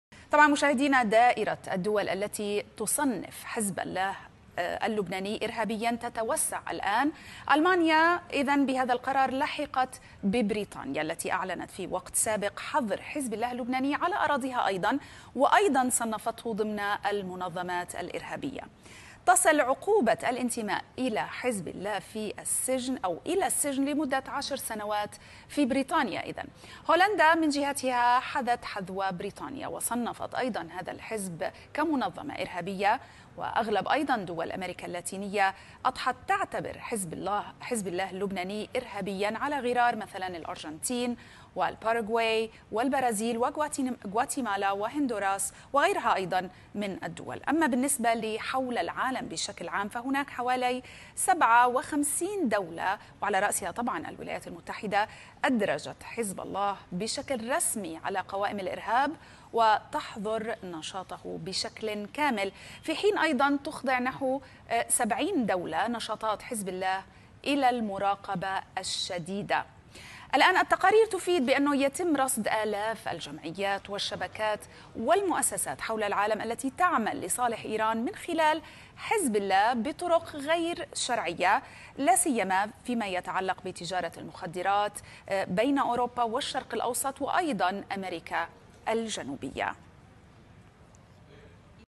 دائرة الدول التي تصنّف حزب الله منظمة إرهابية تتوسّع
Source: alarabiya